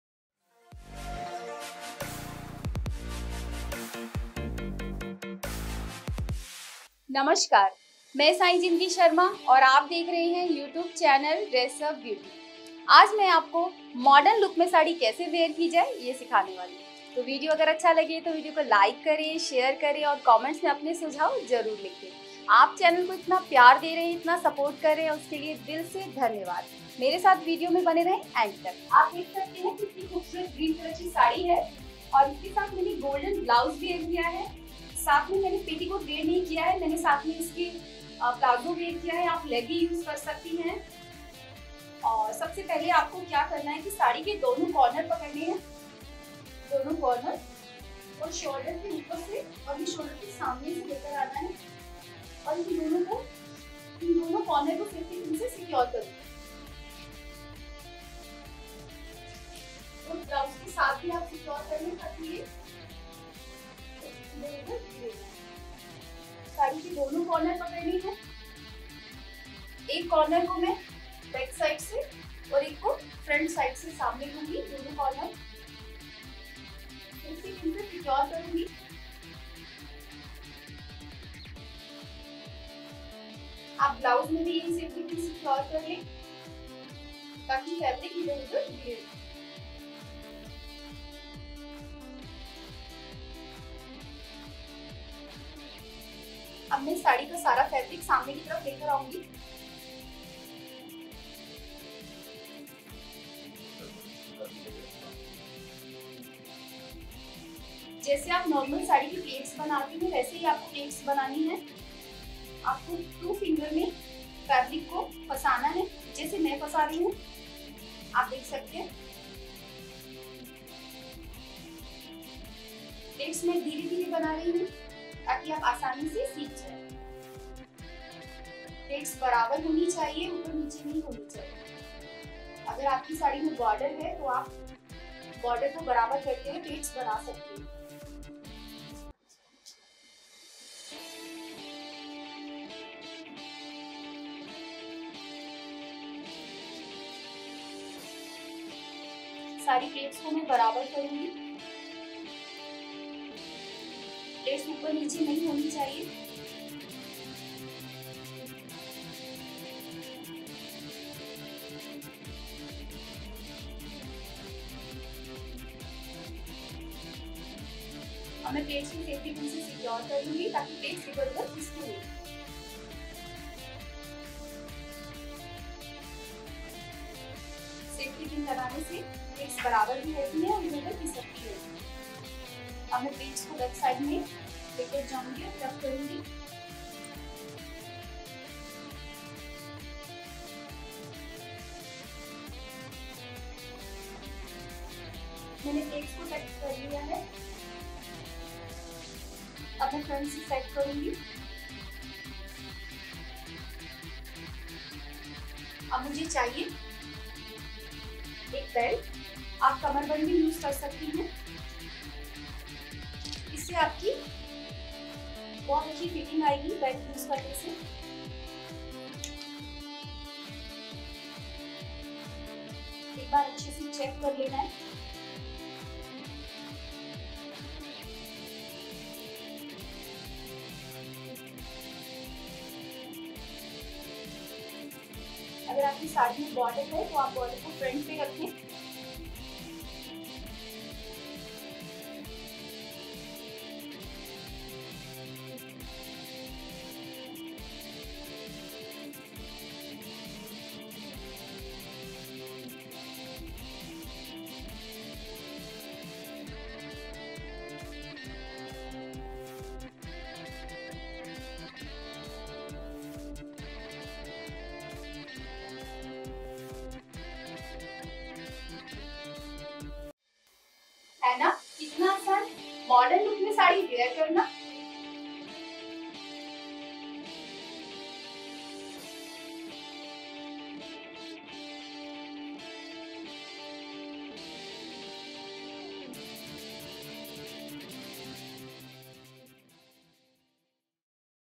New Style Saree Draping in Easy Way | Dress-Up Beauty
Model Name - Sai Jindgi Sharma
Makeup - Raju Hasan
Camera - Anand Bharti
Location - South Delhi
Editor - Rajeev Raj Radheshwari
Directed by Ramesh Sharma